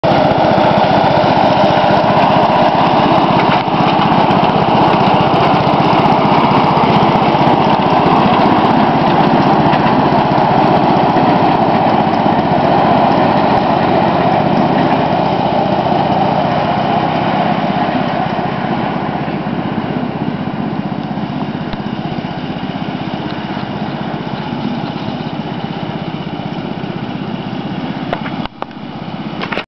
The Swannee River Railroad Loco #5, Test run 1
Jason runs the new geared steam outline on my friends new flat bar style railroad... The tender for #5 is currently being completed. Thecaboose belongs to a friend.